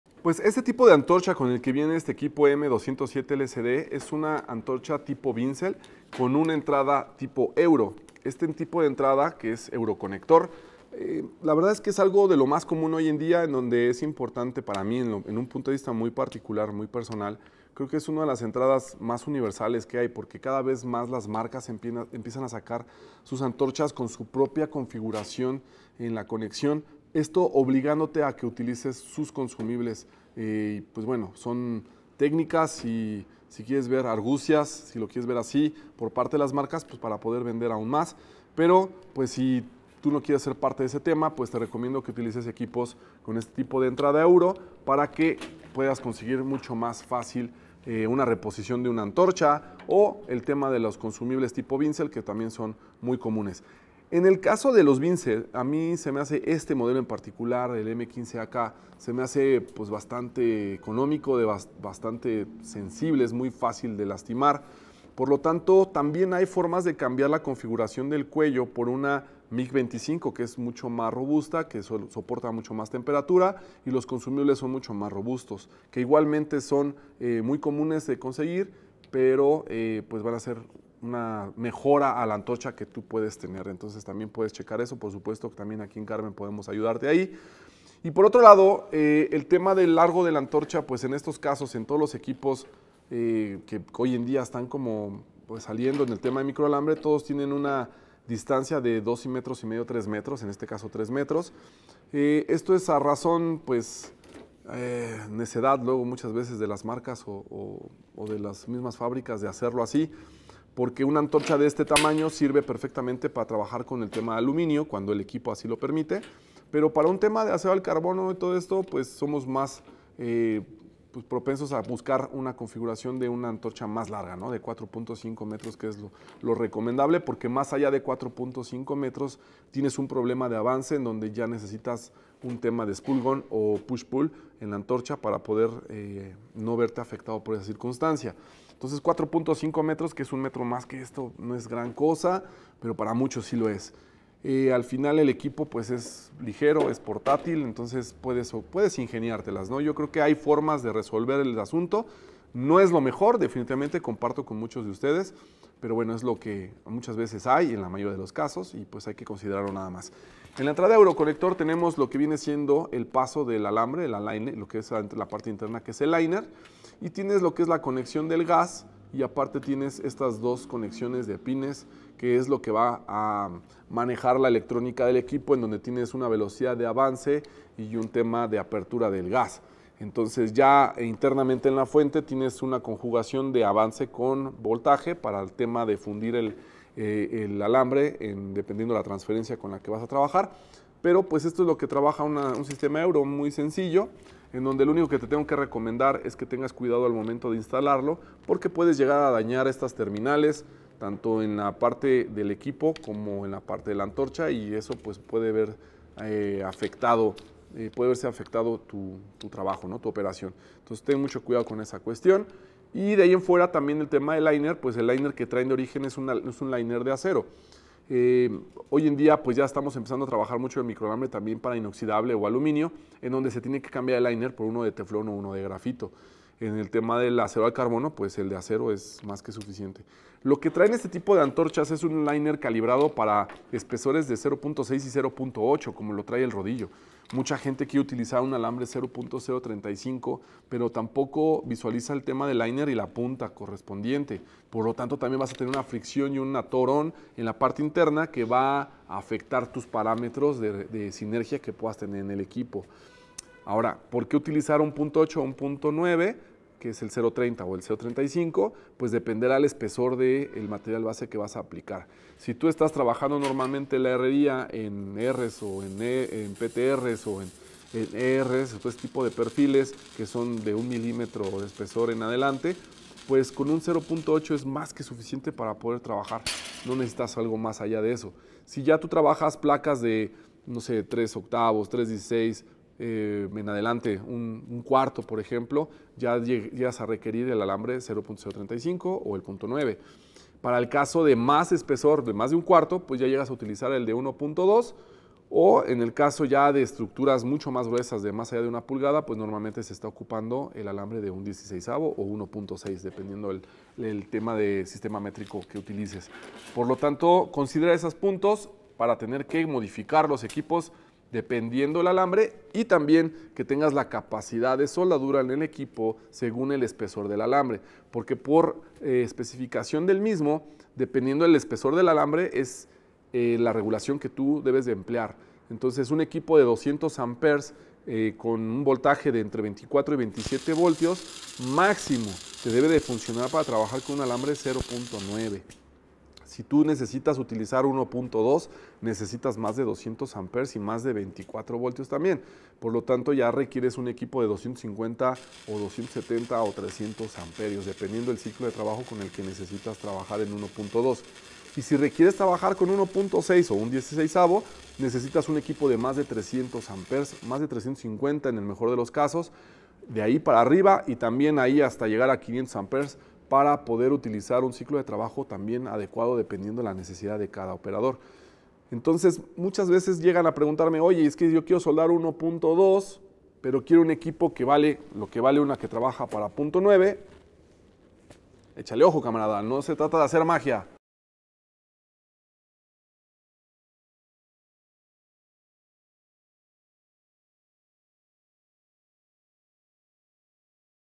Antorcha con euroconector de la AXT-EM207LCD | Jimmy Carben
Analizamos las ventajas de la antorcha con euroconector de la AXT-EM207LCD. En Carben te podemos ayudar a complementar tu equipo para un mejor trabajo.
Manda WhatsApp al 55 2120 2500